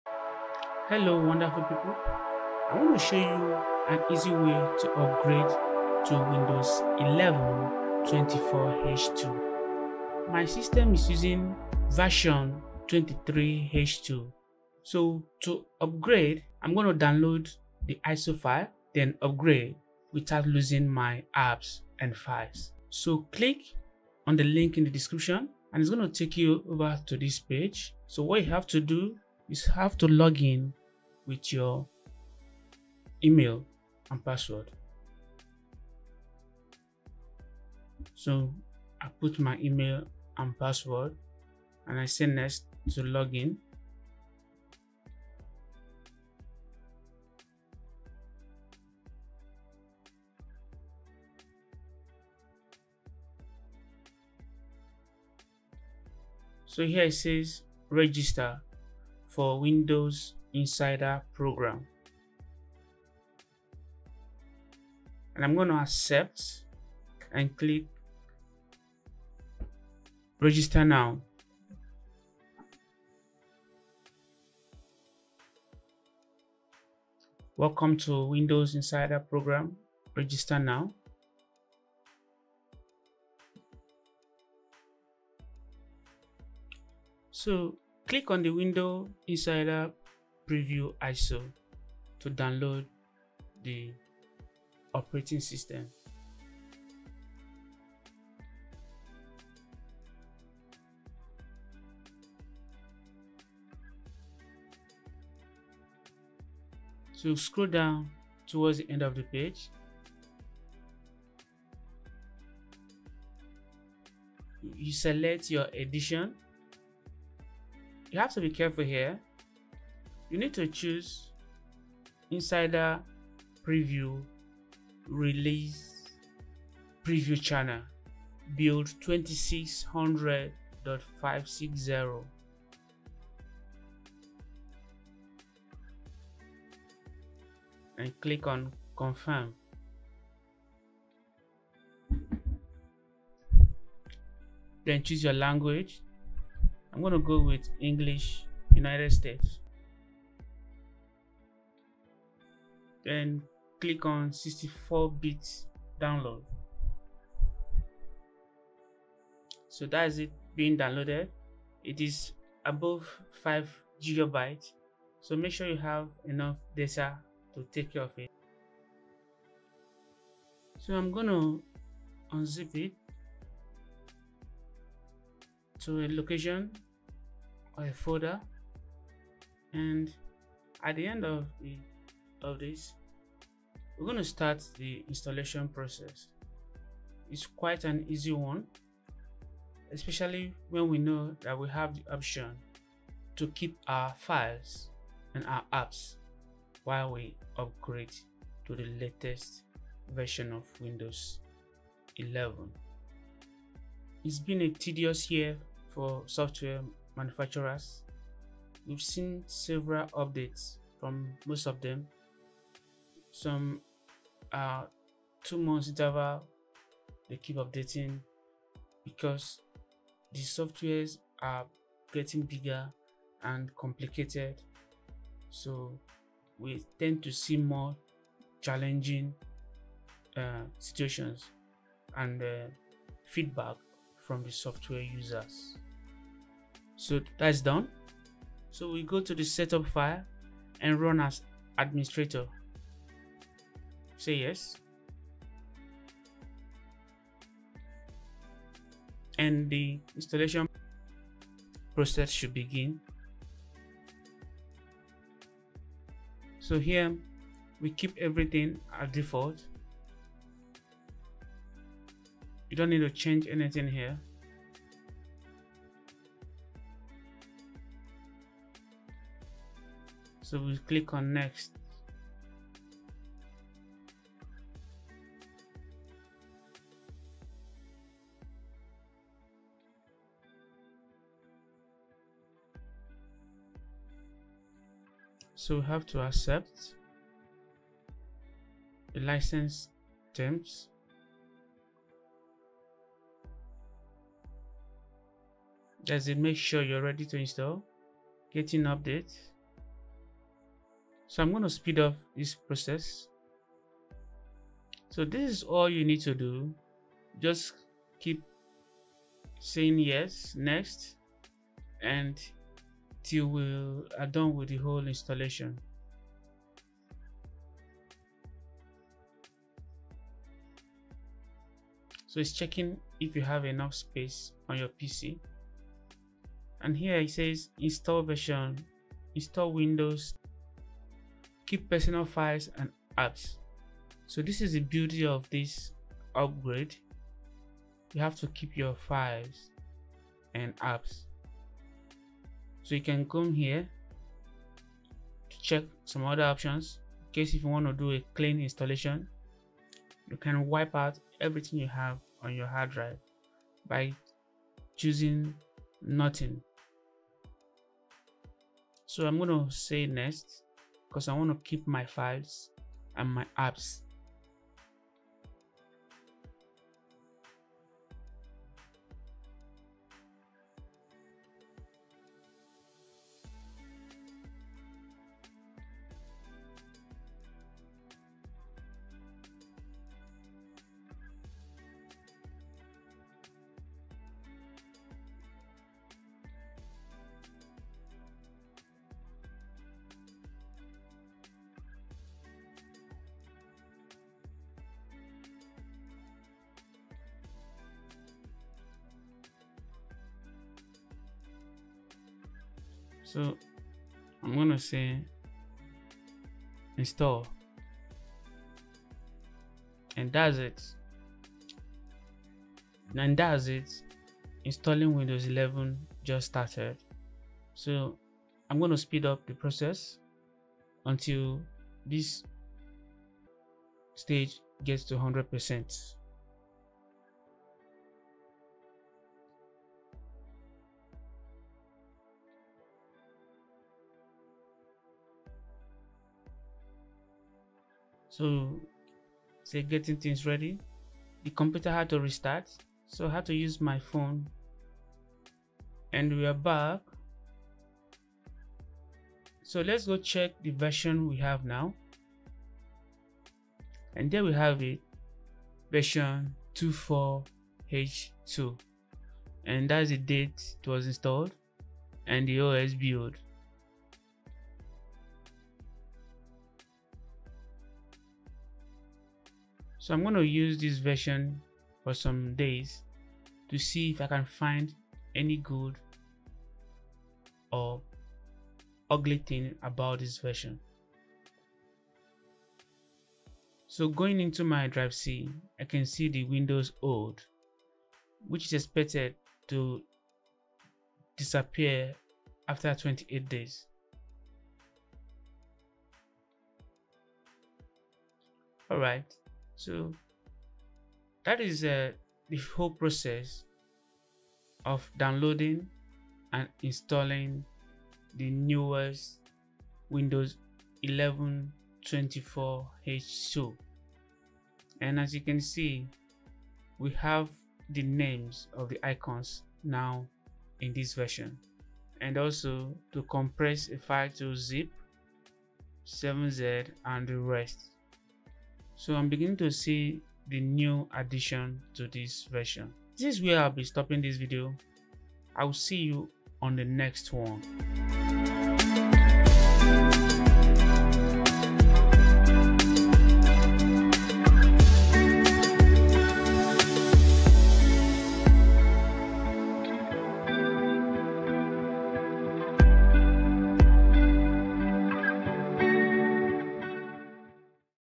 How to Upgrade to Windows 11 24H2 Without Losing Apps and Files | Step-by-Step Guide
Upgrade to Windows 11 24H2 seamlessly without losing any of your apps or files! In this step-by-step tutorial, we guide you through the entire process, ensuring your upgrade is smooth and hassle-free. Whether you're a tech novice or an experienced user, our comprehensive guide covers everything you need to know to make the transition to Windows 11 24H2 easy and efficient.

🔍 In this video, you'll learn:

How to prepare your PC for the Windows 11 24H2 upgrade
Step-by-step instructions for a seamless upgrade
Tips to ensure all your apps and files remain intact
Troubleshooting common issues during the upgrade process
Exploring new features and enhancements in Windows 11 24H2

📌 Why upgrade to Windows 11 24H2?

Enhanced performance and speed
Improved security features
New and intuitive user interface
Exciting new features and tools
Don't miss out on the latest and greatest version of Windows! Follow our guide to upgrade to Windows 11 24H2 today and enjoy a better computing experience without losing any of your important data.